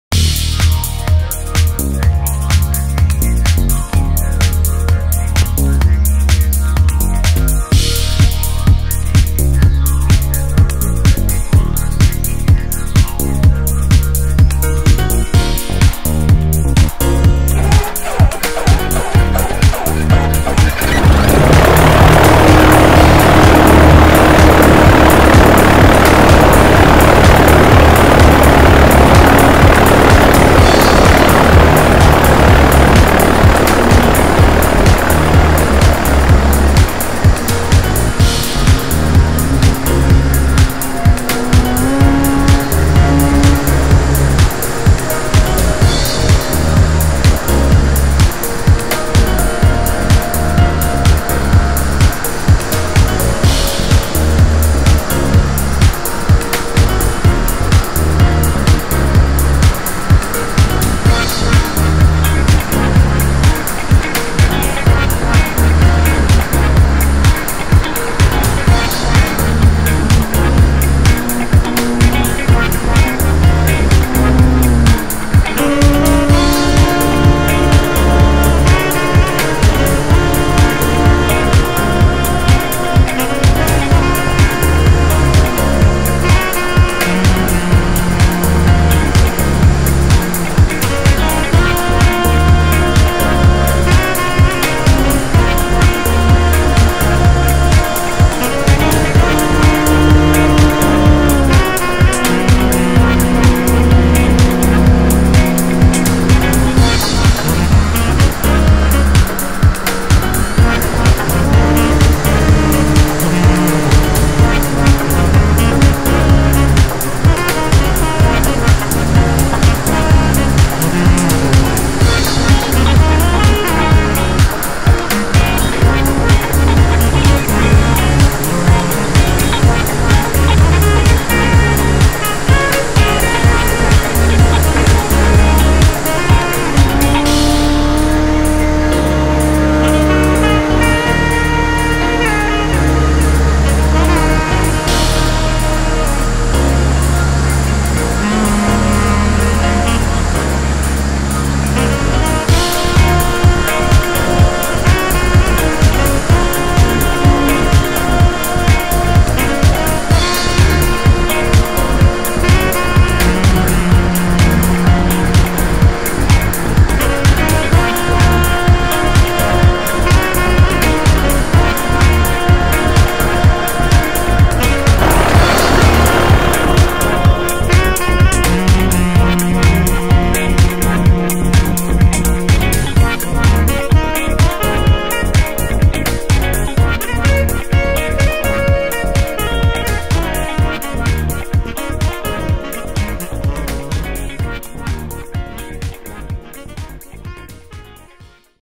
Twinstar follows Heron
One early summer evening I tried to follow with my rc plane "Twinstar 2" another plane. It worked only for seconds. No first person view was available.